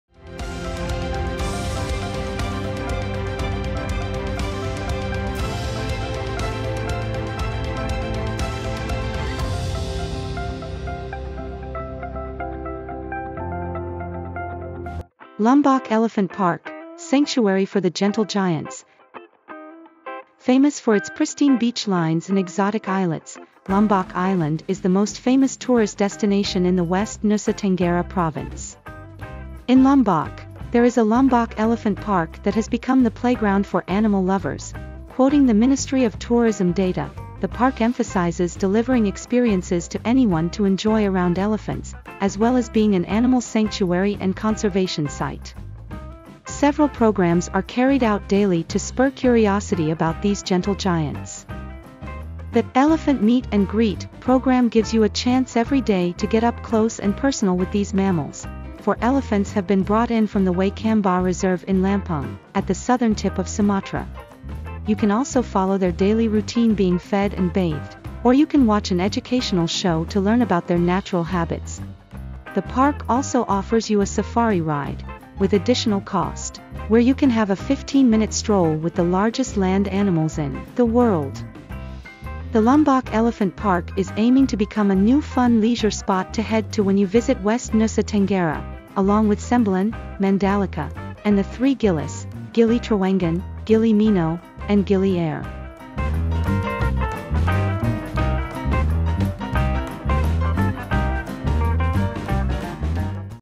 Lombok Elephant Park, Sanctuary for The Gentle Giants
Famous for its pristine beach lines and exotic islets, Lombok Island is the most famous tourist destination in the West Nusa Tenggara province.

In Lombok, there is a Lombok Elephant Park that has become the playground for animal lovers.

Quoting The Ministry of Tourism data, the park emphasizes delivering experiences to anyone to enjoy around elephants, as well as being an animal sanctuary and conservation site.  See more in the video.

Script / Video Editor: Aulia Hafisa / Praba Mustika